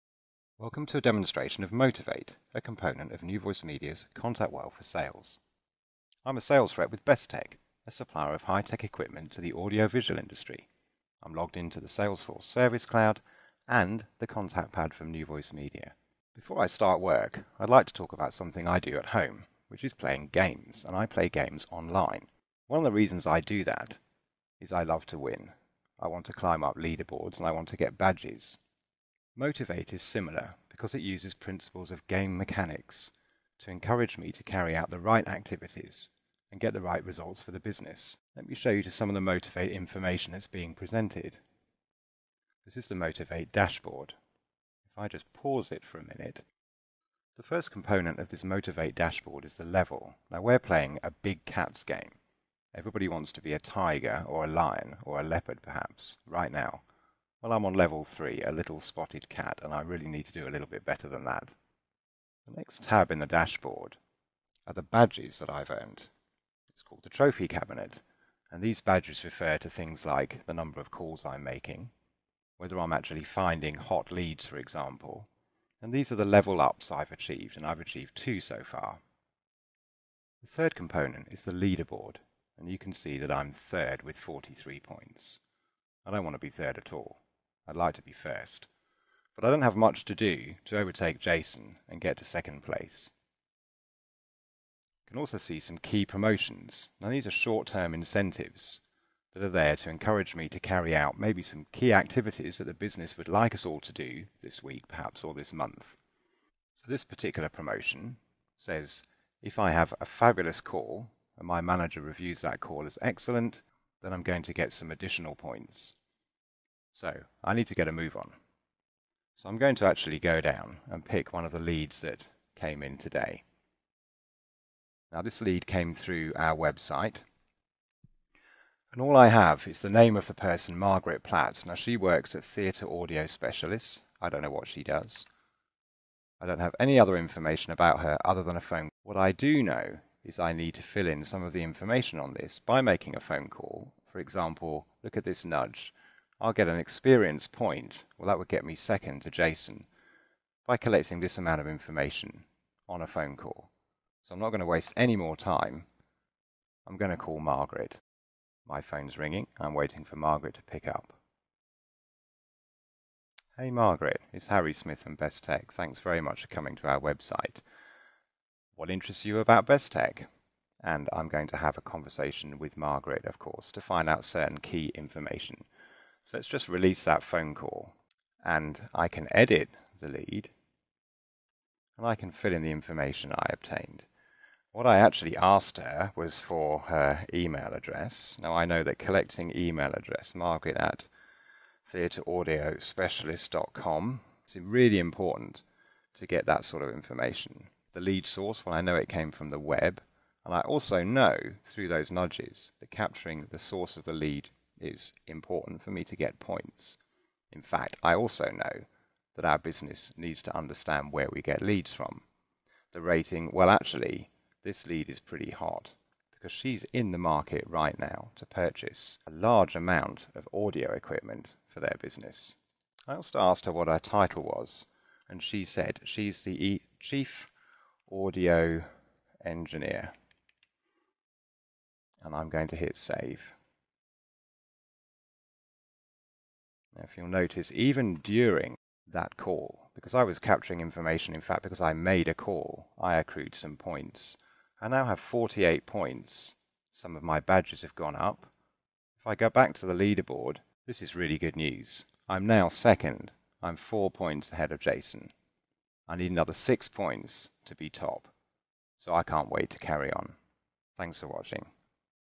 Learn How to Add Inside Sales Gamification to Your Salesforce Org
Learn how to add and use Salesforce gamification to improve the performance of your inside sales team. The Motivate gamification application can easily be added to NewVoiceMedia ContactWorld for Sales. Watch this short video to see how.